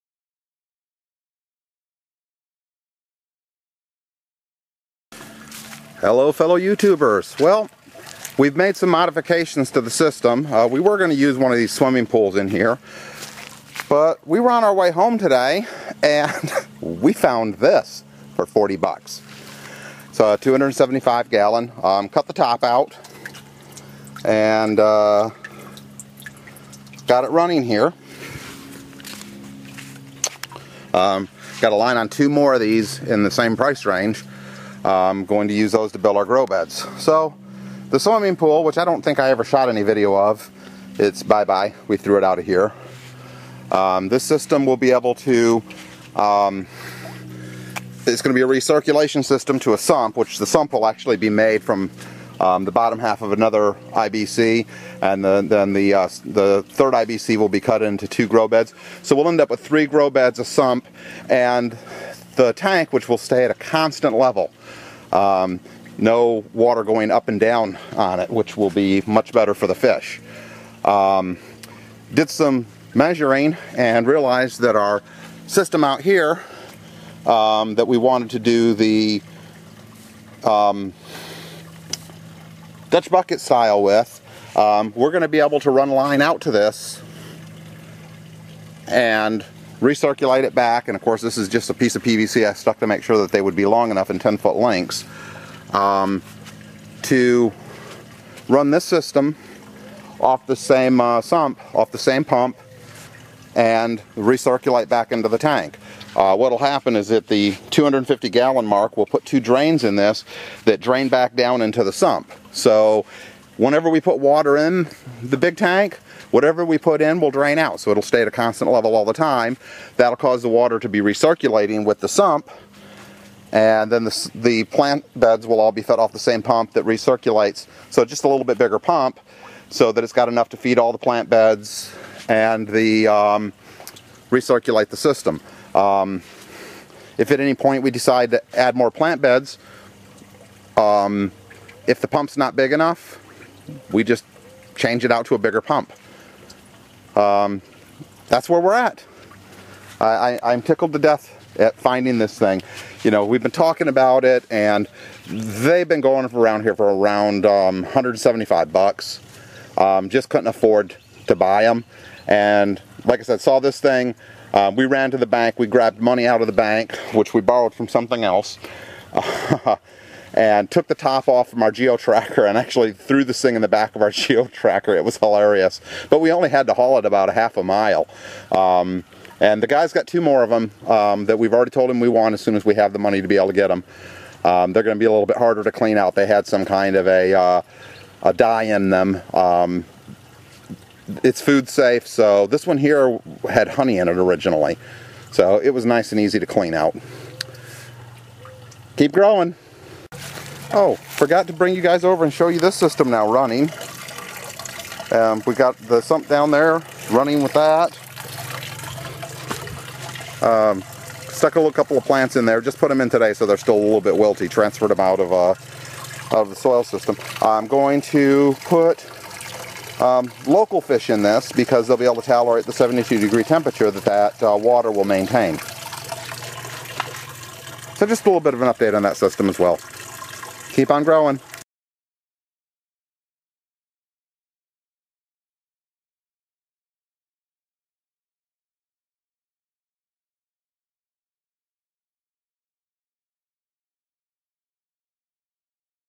Living with the Earth - Faery Gardens 006 - The Fish Tank IBC
WE lucked out and got a deal on this IBC which will now become our fish tank for our Aquaponic system.